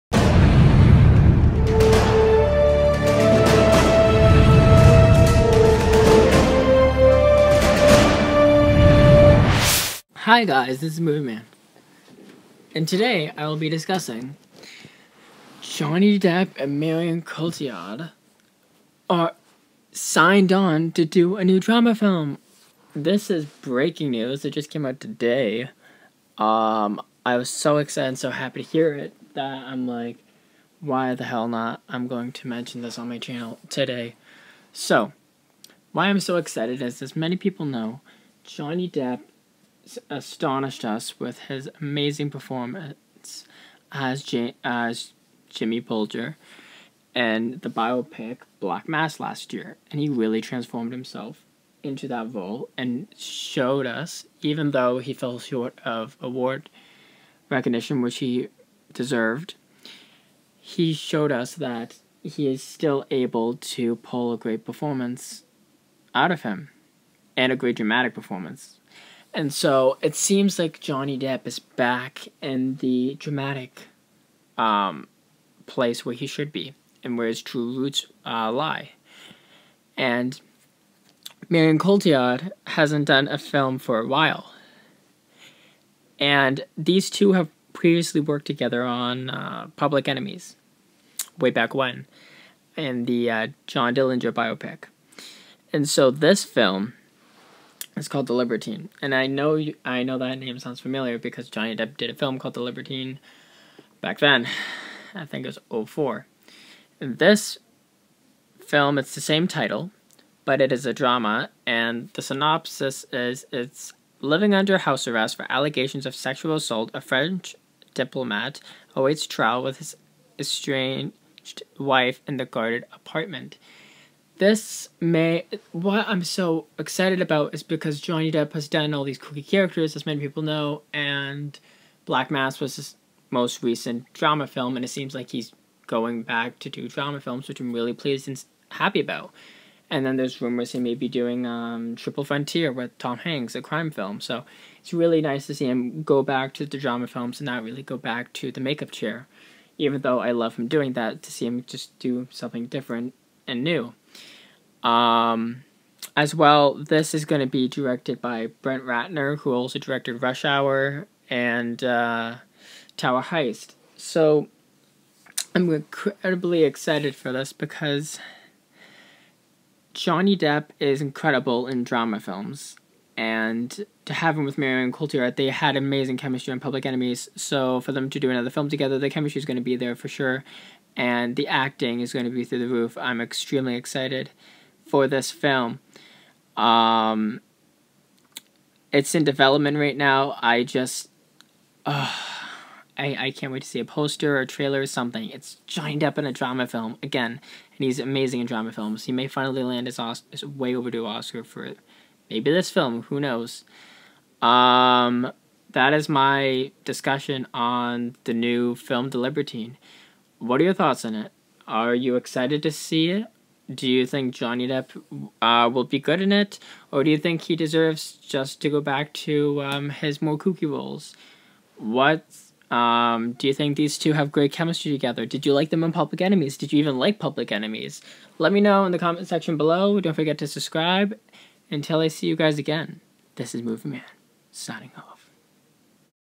MovieMan : Johnny Depp & Marion Cotillard Sign On To A Drama Film
I discuss Johnny Depp & Marion Cotillard newest film they have signed on to do called the libertine